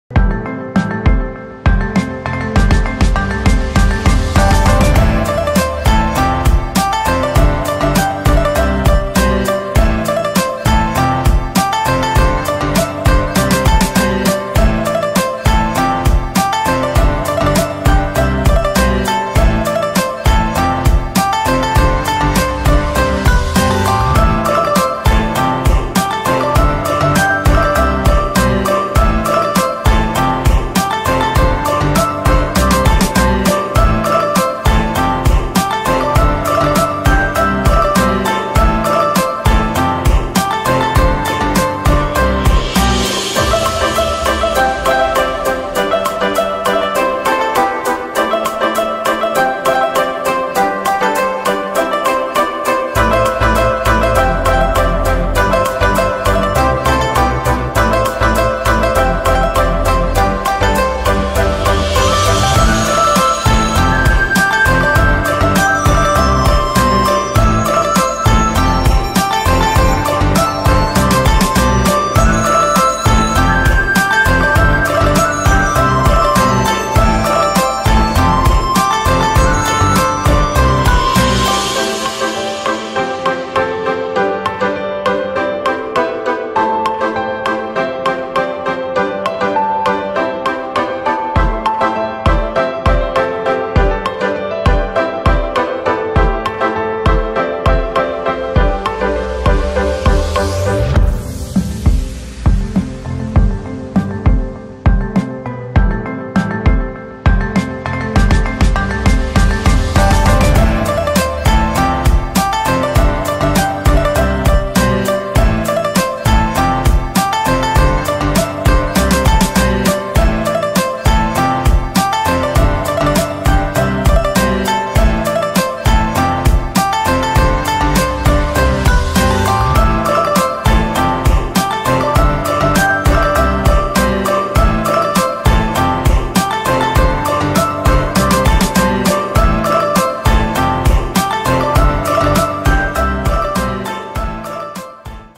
Resep Nasi Ayam Hainan Asli, Ayamnya Empuk, Nasinya Wangi Banget!
Kangen makan Nasi Ayam Hainan di restoran Chinese food langganan? Enggak perlu khawatir, buat dengan resep dari Sajian Sedap, ayamnya empuk, nasinya wangi banget!

00:01- intro
00:07- bahan-bahan nasi ayam hainan
00:10- cara membuat nasi hainan
00:51- cara membuat ayam hainan
01:32- cara membuat kuah nasi ayam hainan
01:56- cara menyajikan nasi ayam hainan

Produser:
Penulis Naskah:
Peraga:
Fotografer: